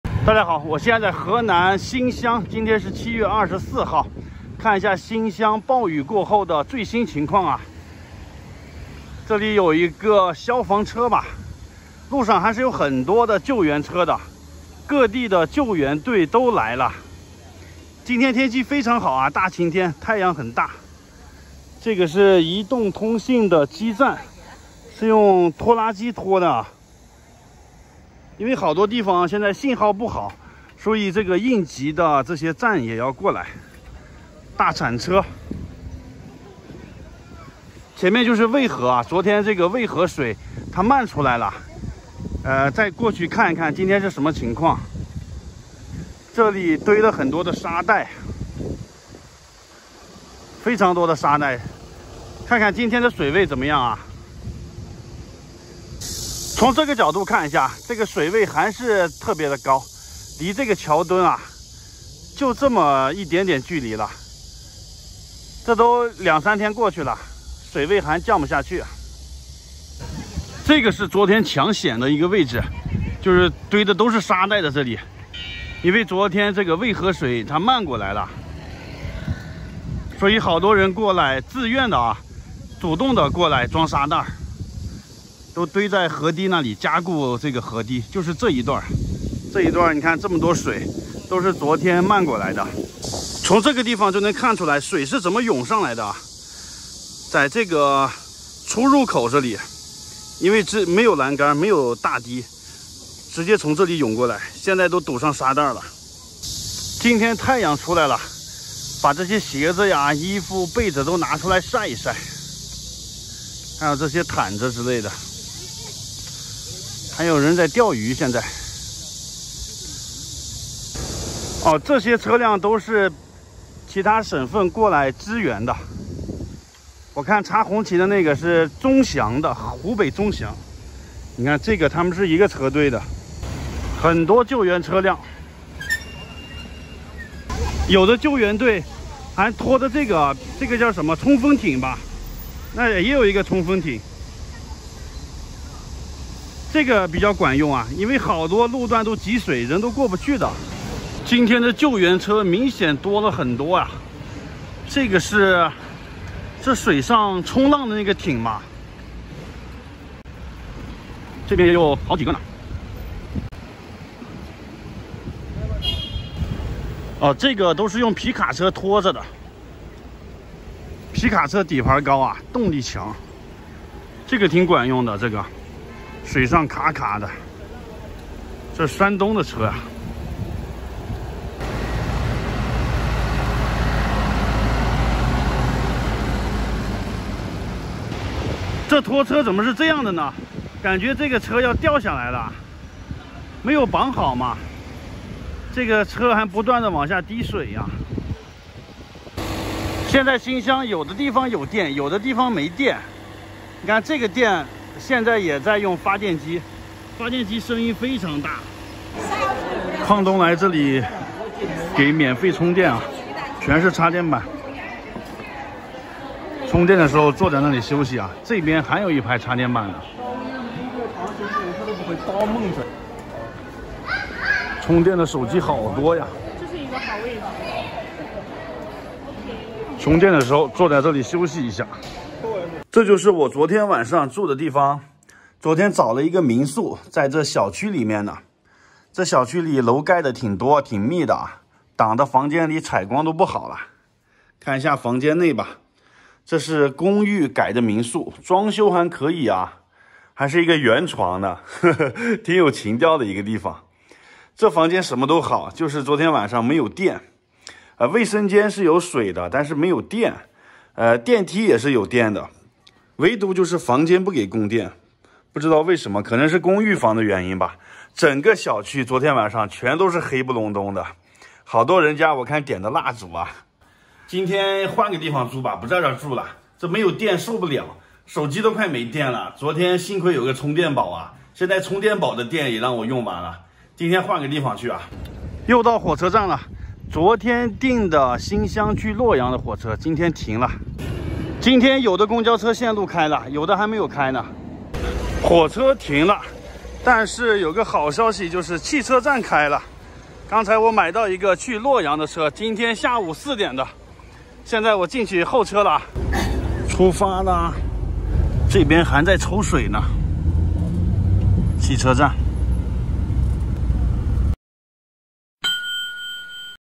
暴雨之后的新乡，全国各地的救援队来了，很多人来帮忙！【大海去旅行】
新乡两天大暴雨，终于放晴了！来看看雨后新乡的样子，来了好多救援队啊！